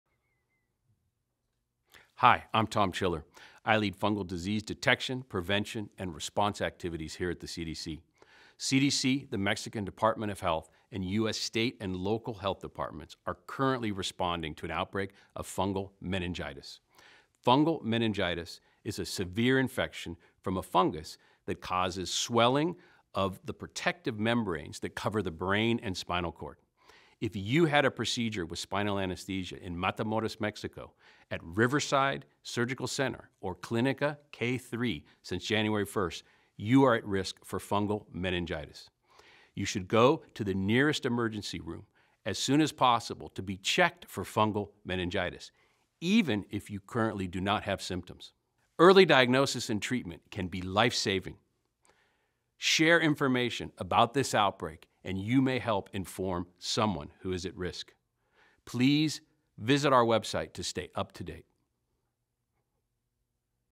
CDC’s Tom Chiller on the Outbreak of Fungal Meningitis - V
CDC, the Mexican Department of Health, and U.S. state and local governments are currently responding to an outbreak of fungal meningitis.